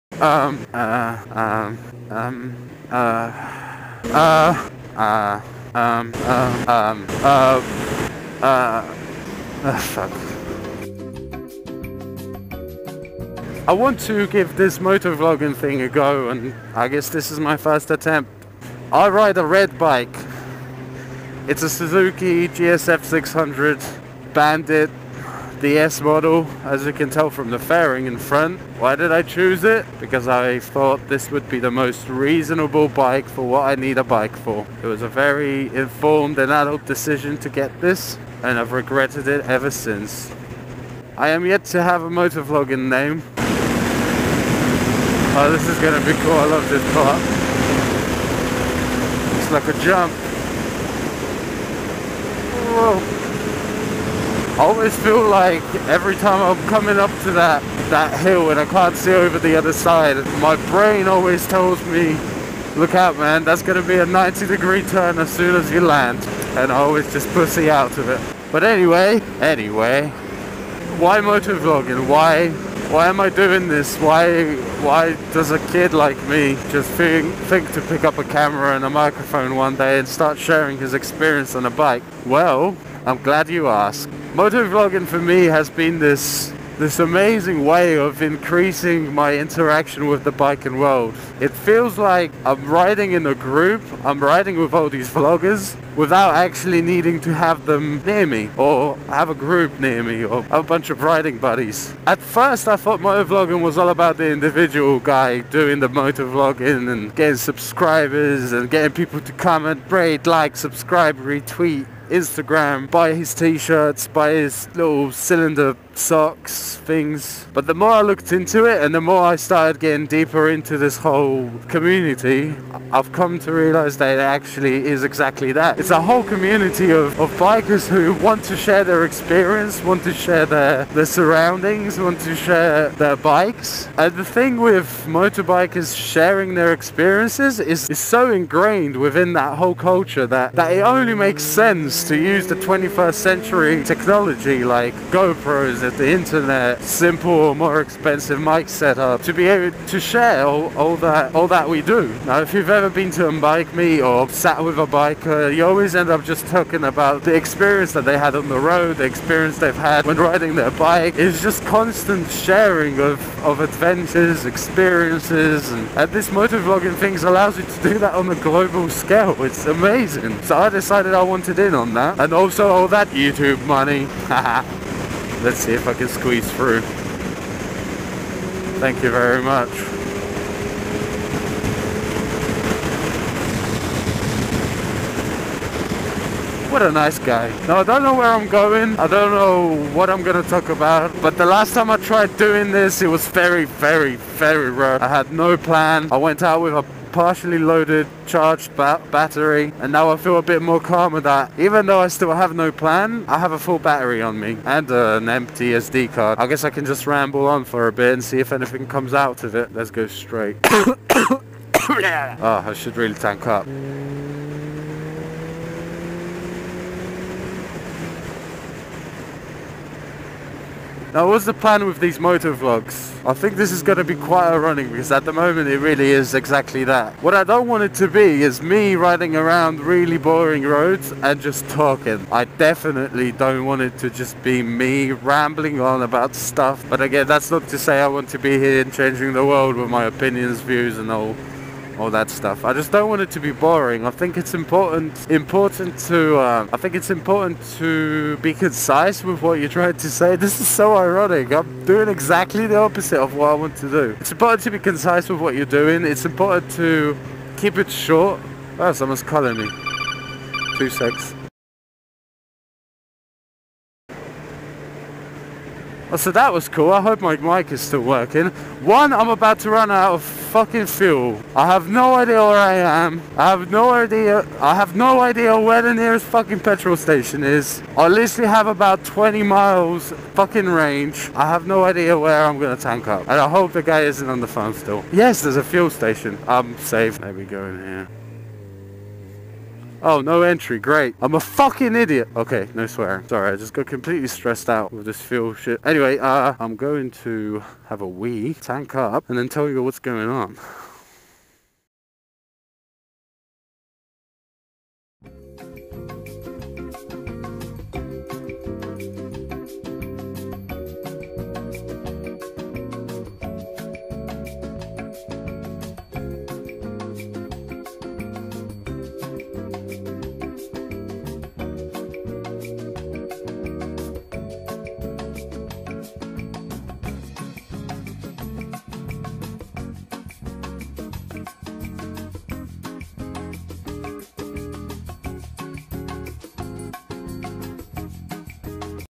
Tash MOTOvlog - Episode 1 - The Beginnings // Awkwardness // What is this
The first video of a new MotoVlogging channel.

Give this a pump up with the thumb up.
It will help me a lot.